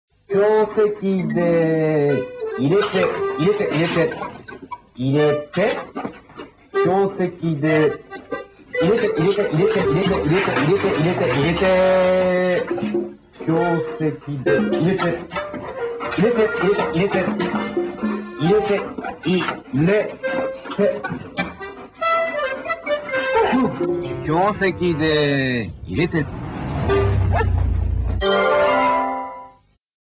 共同石油 TV−CM
共同石油　水飲み犬が登場するTV−CM